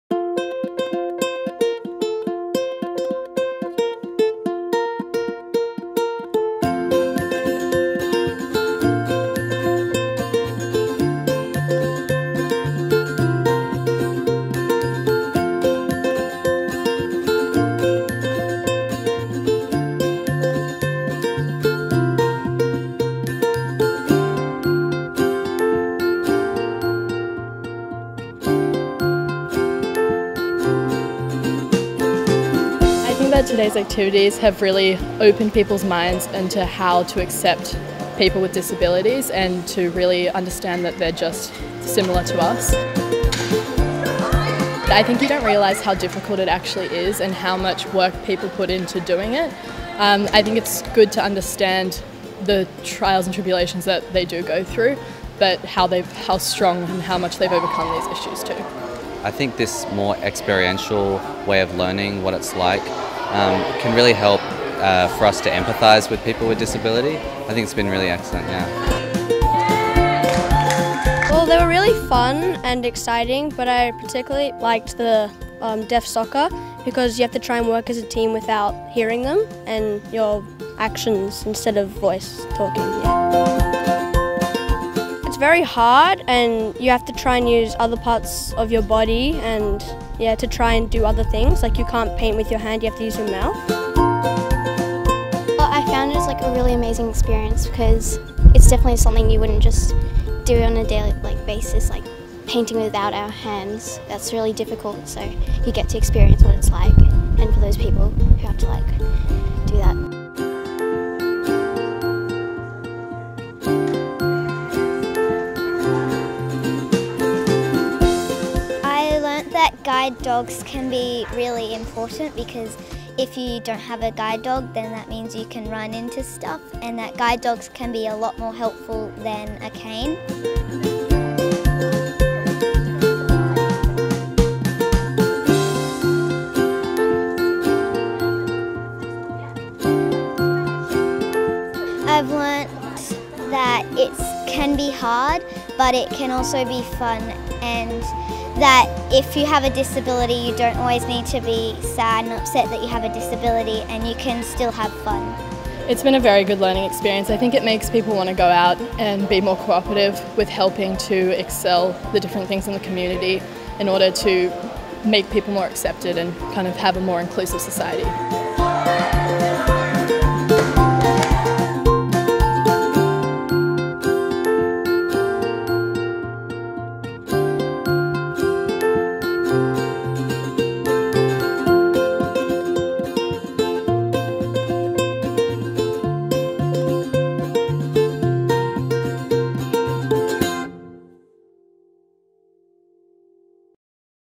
Mosman celebrates International Day of People with Disability 2015
About 140 students from five local schools braved the heat on Thursday 26 November to take part in activities organised by Mosman Council to celebrate International Day of People with Disability (IDPwD).

The event aimed to increase awareness, understanding and acceptance of people with disability whilst celebrating their achievements and contributions.

Students from Mosman Public School, Sacred Heart Primary School, Middle Harbour Public School, Beauty Point Public School and Mosman High School took part in building an inclusive world, wheelchair basketball, painting with your mouth, deaf soccer, Friday Group Art Display and a blindfold challenge.